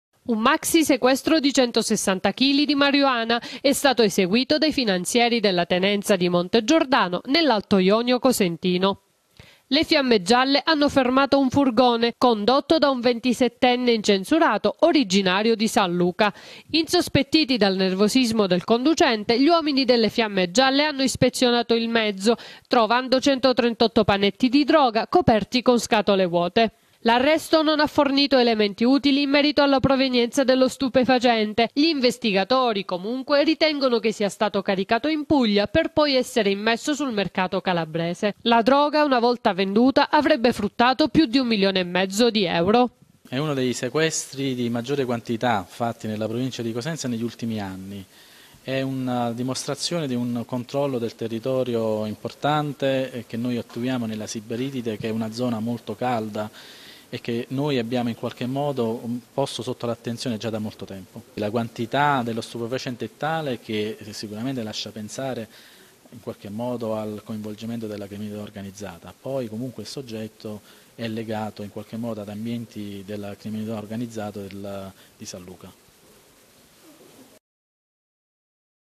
OPERAZIONE DELLA GUARDIA DI FINANZA NEL COSENTINO, SEQUESTRATI 160 CHILI DI DROGA
ra alla guida di un furgone carico di droga. Nel cosentino la Guardia di Finanza ha arrestato un 27enne. Sul mezzo su
cui viaggiava le fiamme gialle hanno trovano 160 chilogrammi di marijuana. La droga avrebbe fruttato 1 milione e mezzo di euro